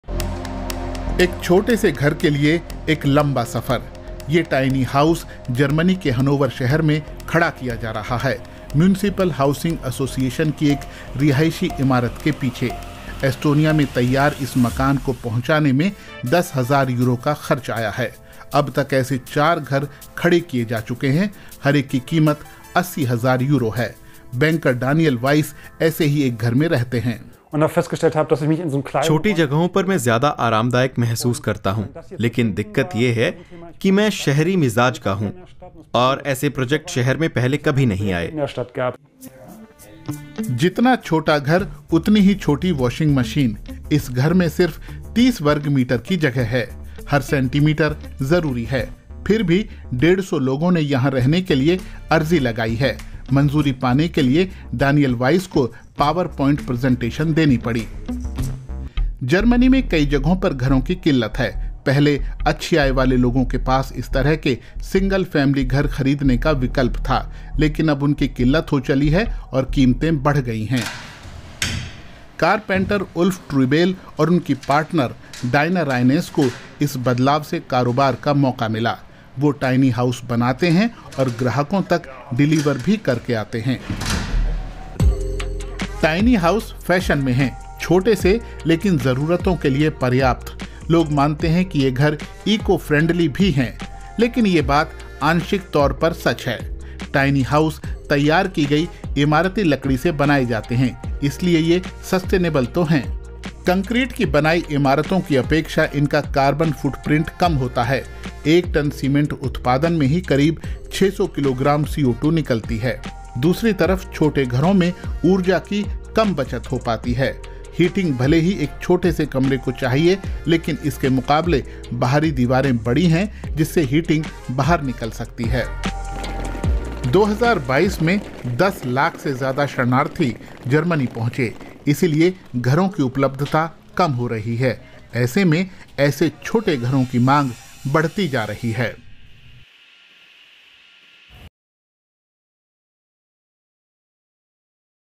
अमीर देशों में अब बड़े नहीं, छोटे घरों का ट्रेंड [Are tiny homes a solution for urban housing?]
शहरी इलाकों में बढ़ती आबादी और घरों की कमी से निपटने के लिए छोटे मकान मददगार हो सकते हैं. इन्हें बनाने में खर्च भी कम आता है और वक्त भी कम लगता है. और, क्या यह विकल्प सस्टेनेबल भी है या नहीं?

With urban populations exploding and a shortage of housing in most cities, alternative solutions are needed. Could tiny homes provide one answer? They're compact, quick to build and cost less than a traditional home. Are they also more sustainable?